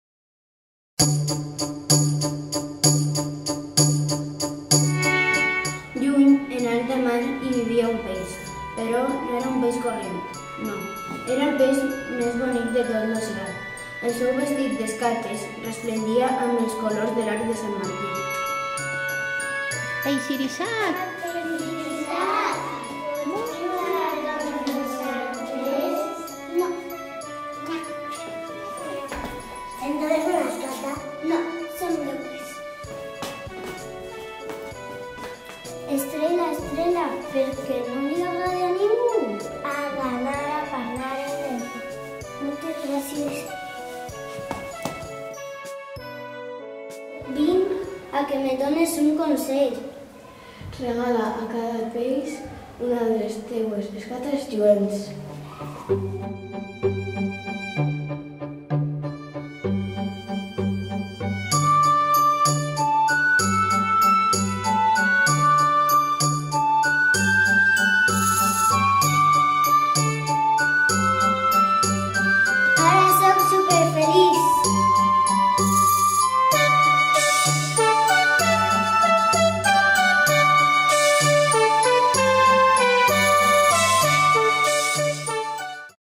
MICE_VAL_BKTINFPRI_56 ''EL PEIX IRISAT''
Alumnat de 3 Anys, 4 Anys, 5 Anys d'Infantil 1er, 2on, 3er, 4t, 5é, 6é de Primària
CRA EL SABINAR (Aulari de Alpuente). Alpuente. València.
Irisat és el peix més bonic de l'oceà. Però malgrat la seua bellesa es sent molt sol. Per què els altres peixos no volen jugar amb ell?